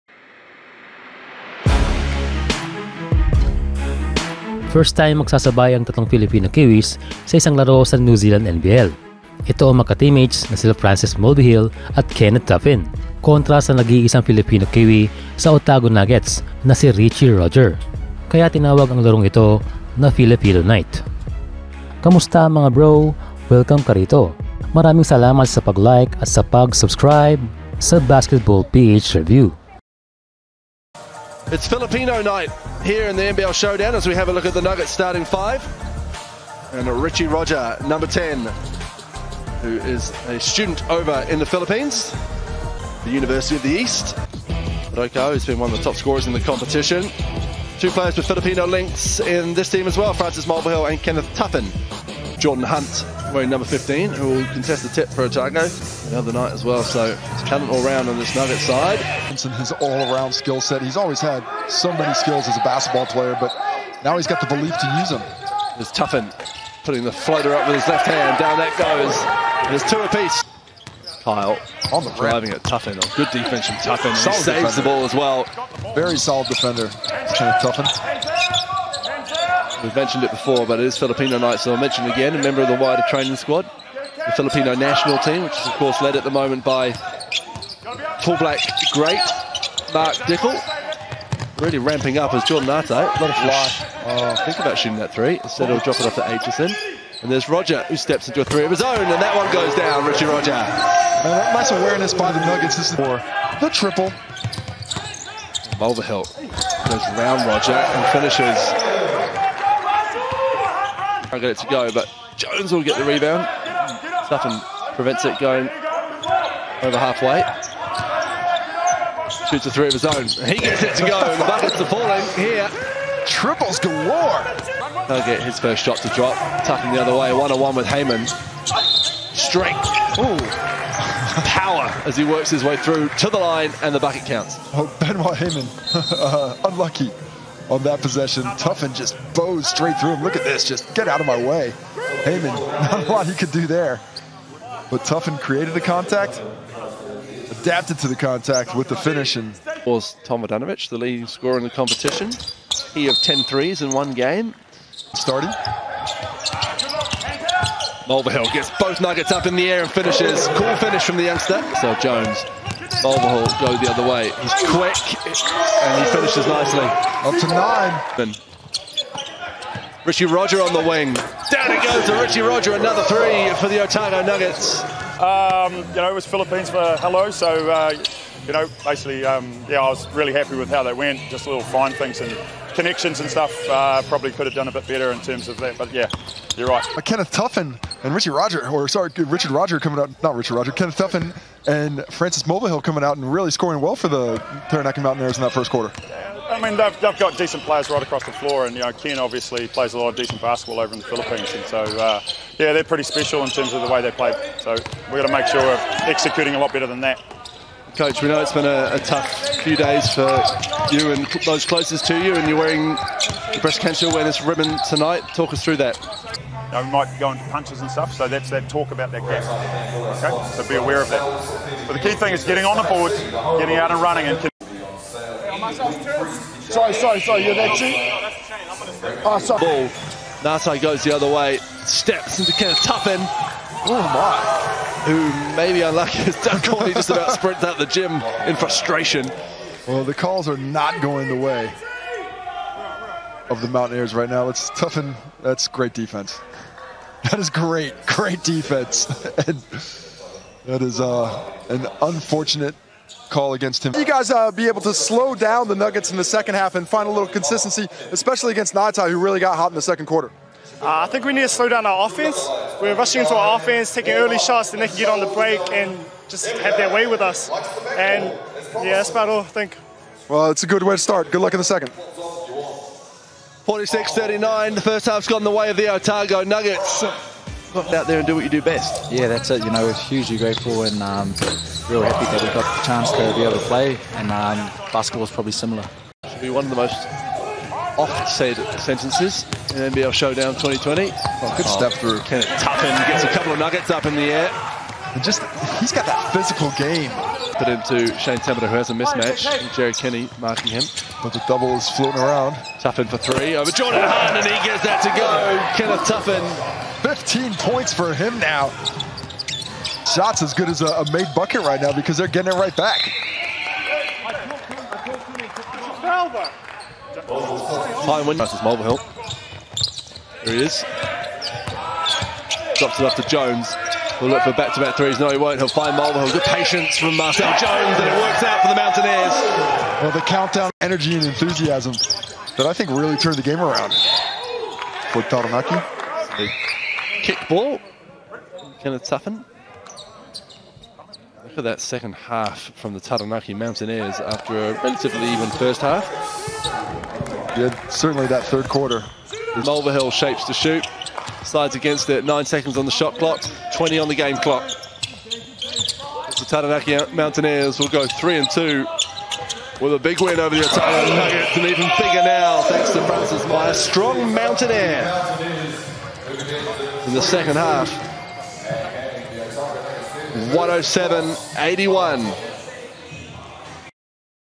Laro ng NZ NBL para sa mga Pinoy | Nagpasiklab sina Francis Mulvihill , Richie Roger at Ken Tuffin
Kamusta mga Bro at Sises